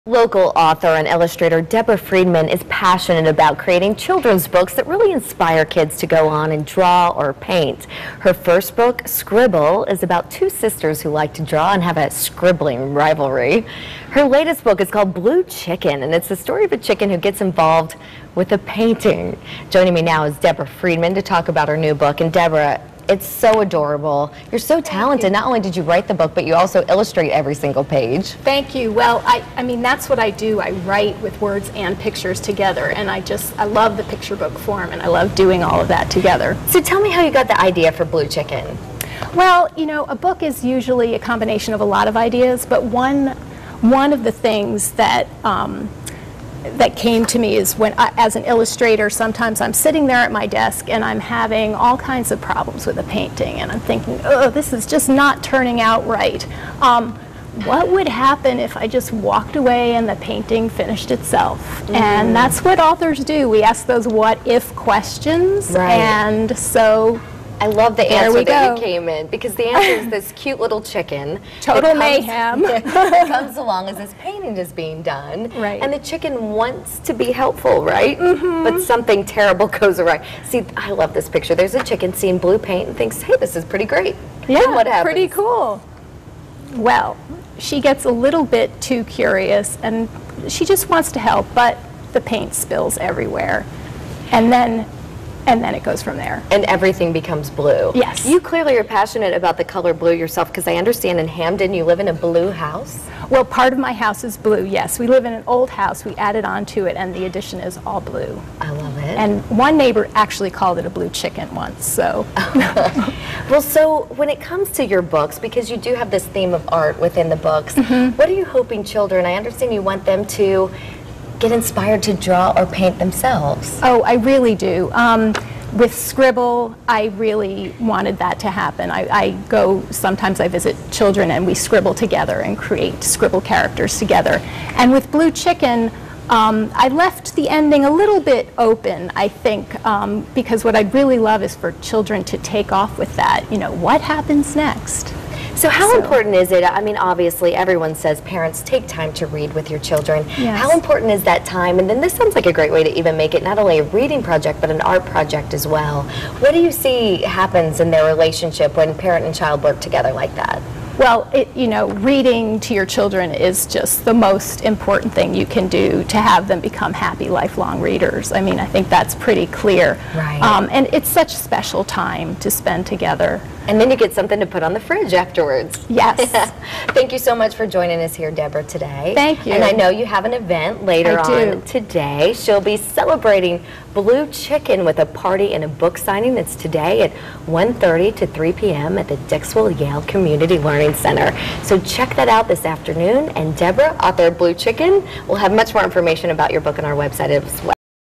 Deborah Freedman's "Blue Chicken"
The idea for Blue Chicken actually came to me while I was reading William Carlos Williams's poem, Red Wheelbarrow.